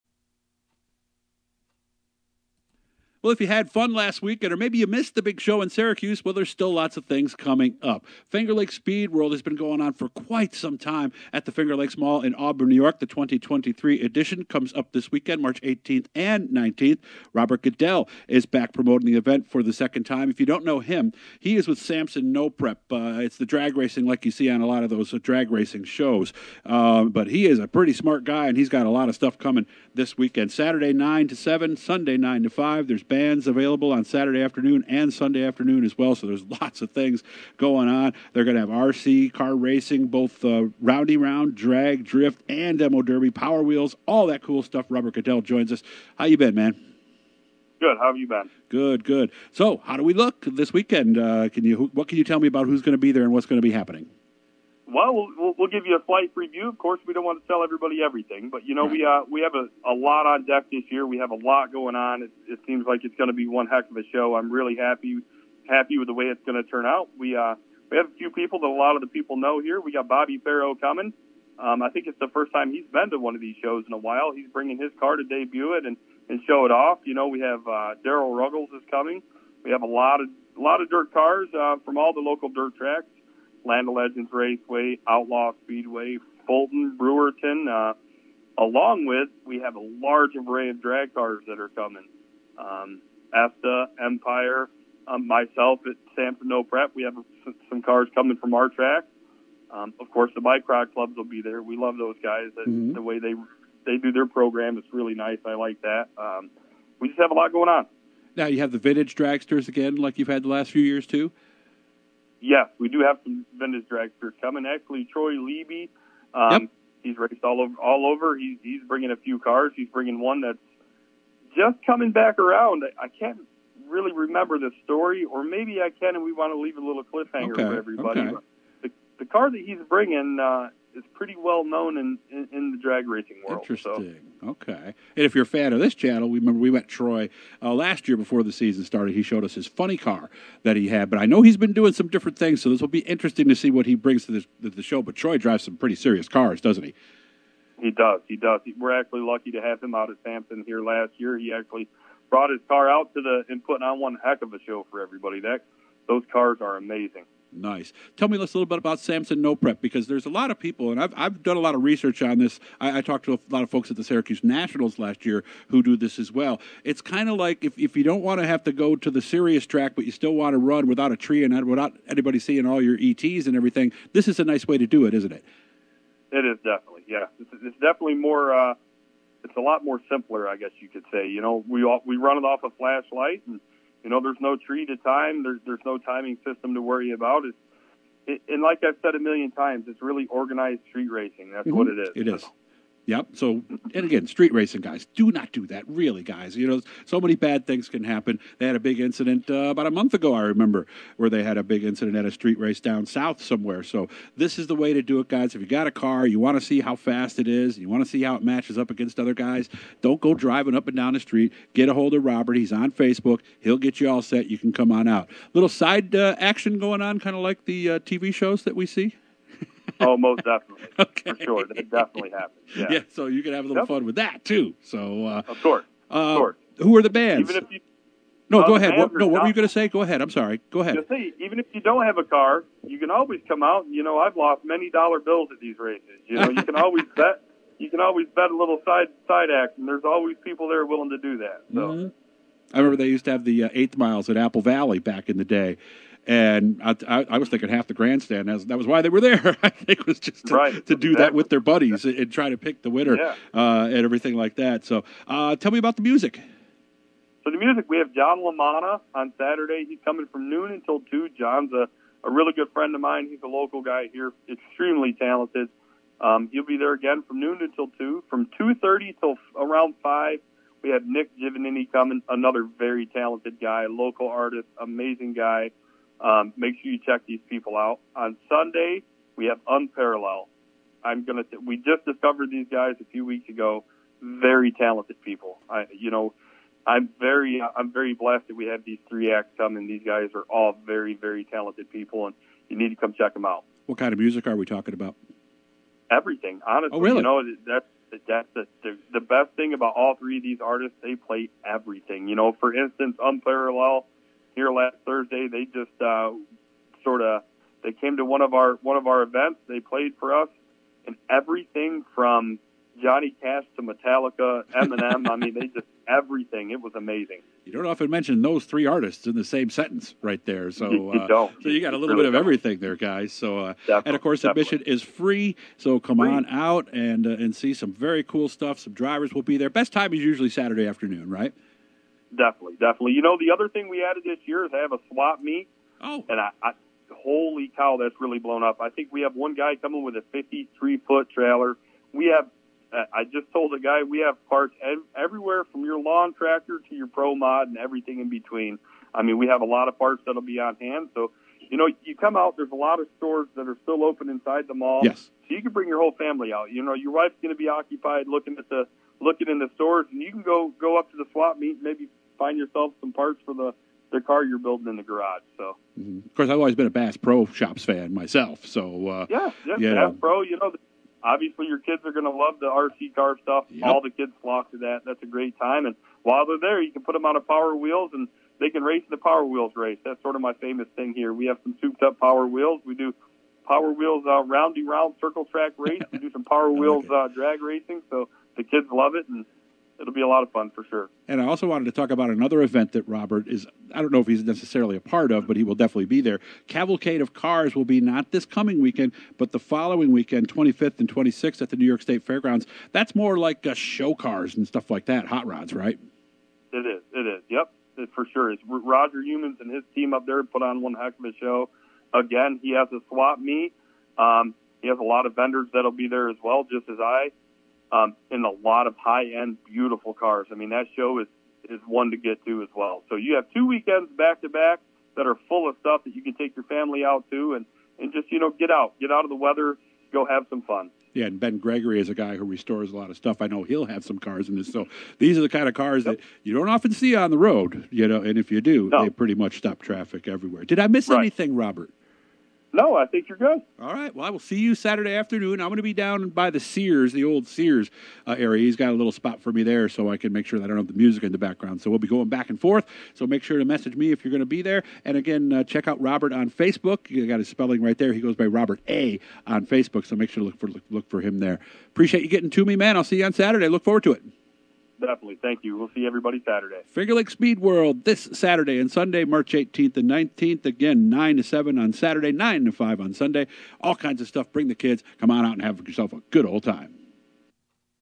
Robert Goodell previews Fingerlakes Speedworld Mar 15 2023
Robert Goodell previews Fingerlakes Speedworld taking place this Saturday and Sunday inside the Fingerlakes Mall in Auburn, New York, tells me some of the different kinds of racecars that will be on display as well as the various activities taking place. I'll see you there this Saturday afternoon.